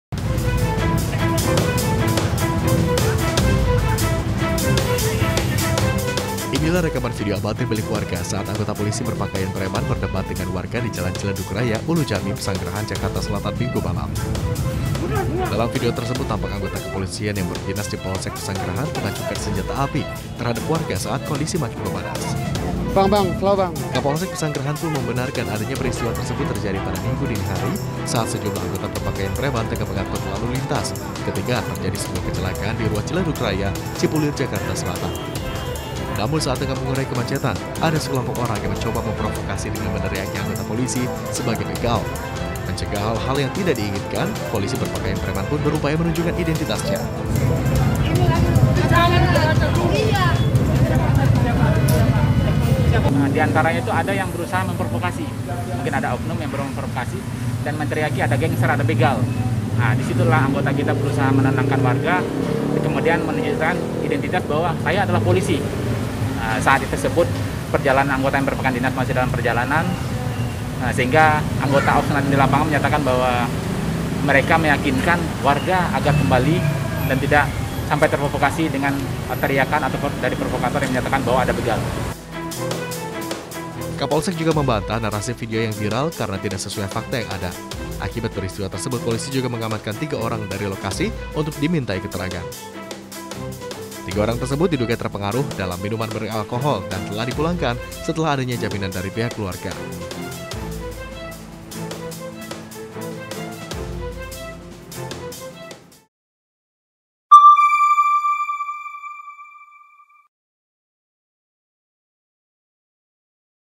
Anggota Polisi Acungkan Pistol saat Urai Kemacetan di Jalan Ciledug Raya, Ternyata Ini Motifnya
Anggota Polsek Pesanggrahan berpakaian preman terlihat mengacungkan pistol saat berusaha mengurai kemacetan di Jalan Ciledug Raya, Ulujami, Pesanggrahan, Jakarta Selatan.

Android: inews.id/android
iPhone: inews.id/ios